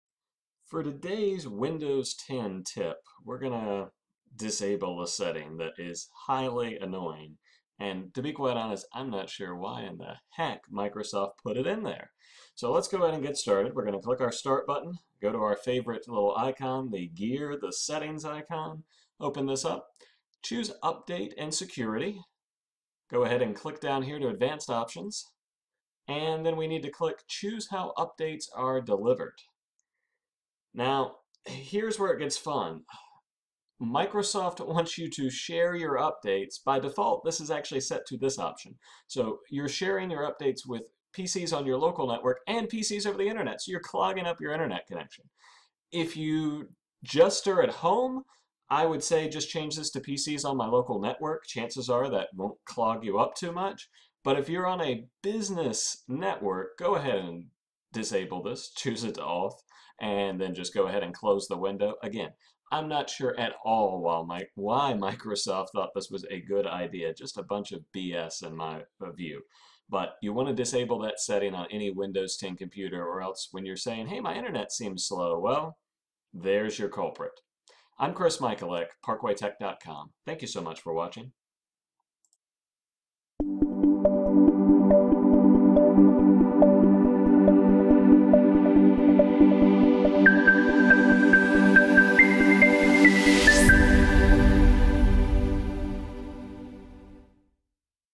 Windows 10 - Disable Update Sharing
You MUST do this if you have a Windows 10 computer. Don't allow Windows 10 to clog up your Internet connection. Fix it in just 60 seconds!